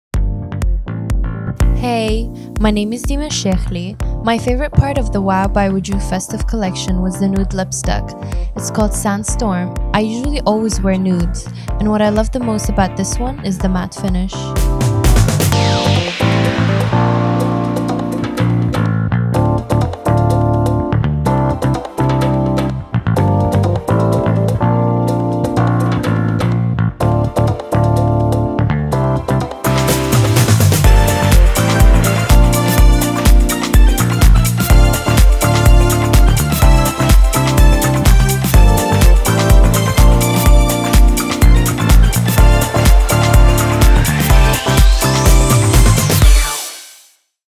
Wow by Wojooh - Festive Makeup Look with Dima Al Sheikly
After an extremely crazy year for Wow by Wojooh – we had to end with a bang! A brand new 15 piece Festive Collection, perfect for any party look.

We gathered some of our favourite Wow by Wojooh girls and asked them - what look will you be sporting this festive season? Dima Al Sheikly - our Dubai socialista - went for a trendy nude lips with big focus on her eyes!